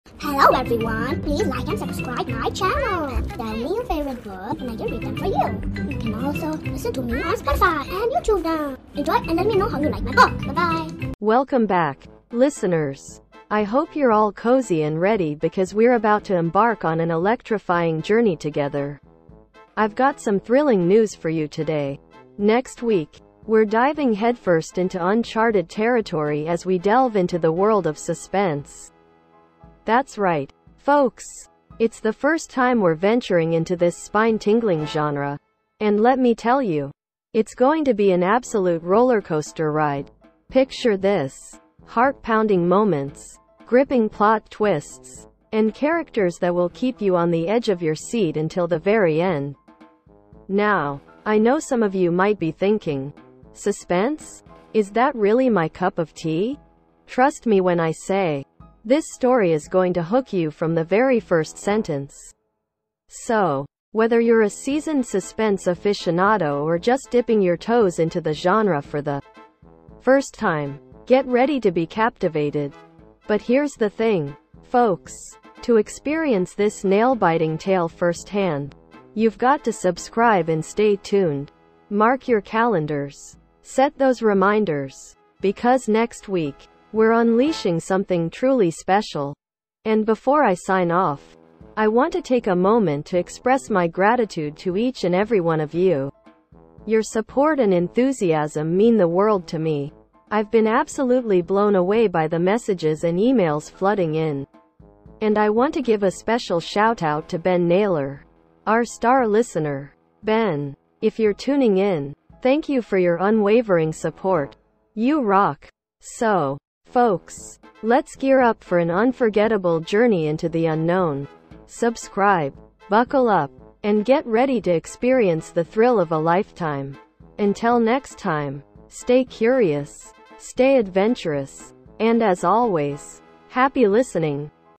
Exciting news about upcoming episodes!! Hold your breath!!
Welcome back, listeners! I hope you're all cozy and ready because we're about to embark on an electrifying journey together. I've got some thrilling news for you today. Next week, we're diving headfirst into uncharted territory as we delve into the world of suspense!

That's right, folks! It's the first time we're venturing into this spine-tingling genre, and let me tell you, it's going to be an absolute rollercoaster ride. Picture this: heart-pounding moments, gripping plot twists, and characters that will keep you on the edge of your seat until the very end.

Now, I know some of you might be thinking, Suspense? Is that really my cup of tea? Trust me when I say, this story is going to hook you from the very first sentence. So, whether you're a seasoned suspense aficionado or just dipping your toes into the genre for the first time, get ready to be captivated.

But here's the thing, folks. Mark your calendars, set those reminders, because next week, we're unleashing something truly special.

And before I sign off, I want to take a moment to express my gratitude to each and every one of you. Your support and enthusiasm mean the world to me. I've been absolutely blown away by the messages and emails flooding in, and I want to give a special shoutout to Ben Naylor, our star listener. Ben, if you're tuning in, thank you for your unwavering support. You rock!

So, folks, let's gear up for an unforgettable journey into the unknown. Until next time, stay curious, stay adventurous, and as always, happy listening!